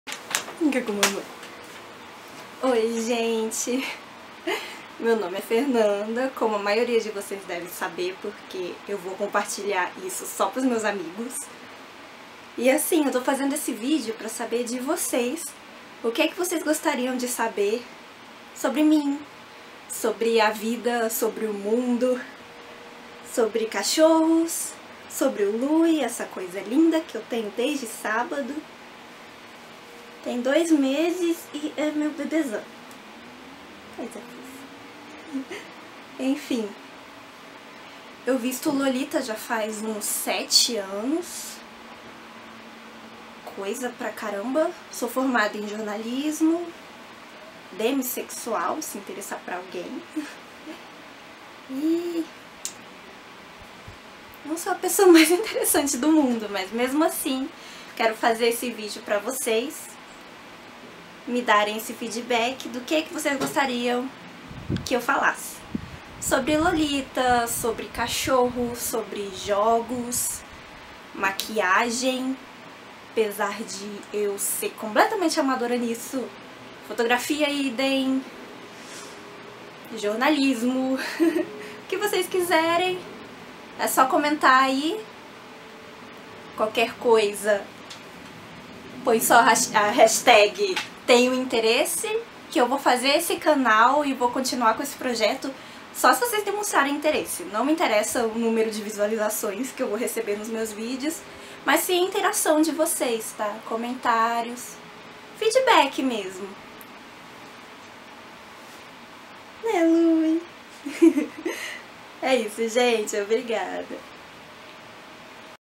Projeto Experimental: Should I open a YT channel?!
Use a hásh-hashtag meinteresso, se quiser que esse projeto de canal saia do papel (';
Aproveita e comenta: Sobre o quê quer que eu fale nos próximos vídeos?

Também aceito sugestões de nomes ♥